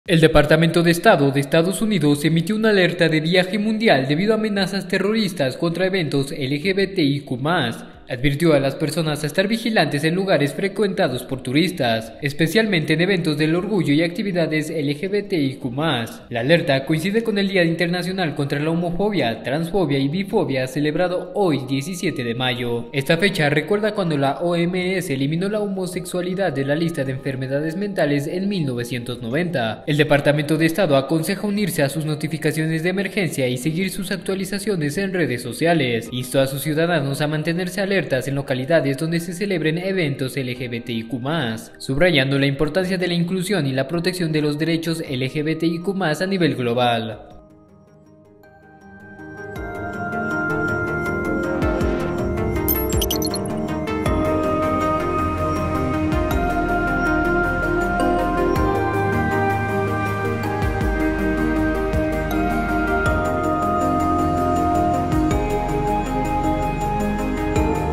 EU emite alerta de viaje mundial por amenazas terroristas en eventos LGBTQI+
El Departamento de Estado llamó a sus ciudadanos a mantenerse alerta en localidades donde se celebren eventos de la comunidad LGBTIQ+.